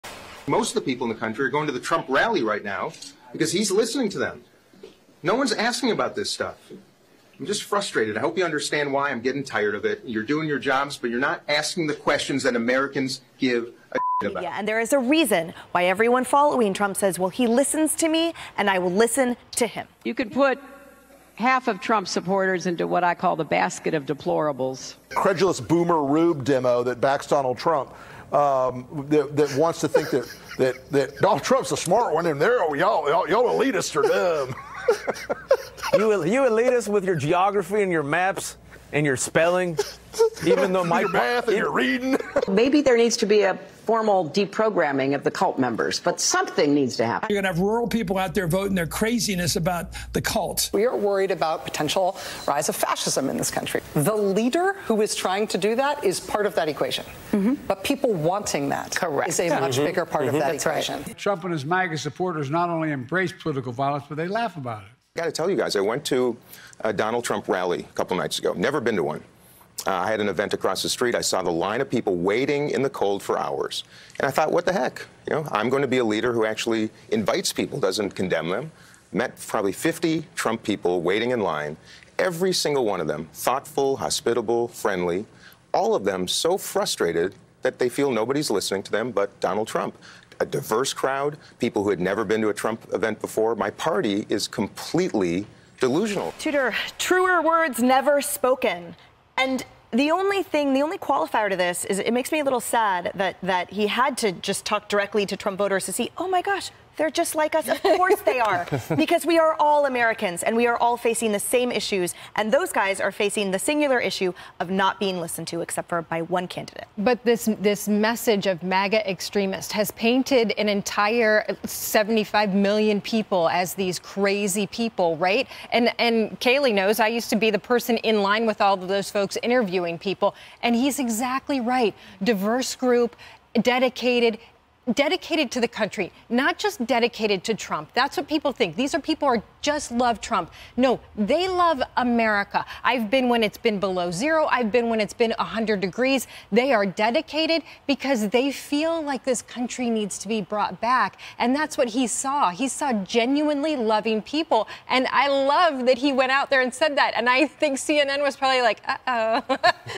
Why Trump is the best in 2024!!
Democrat Challenger goes to Trump Rally..Republicans are loyal to America and just want their voice heard. Trump listens!  @awakeyet3594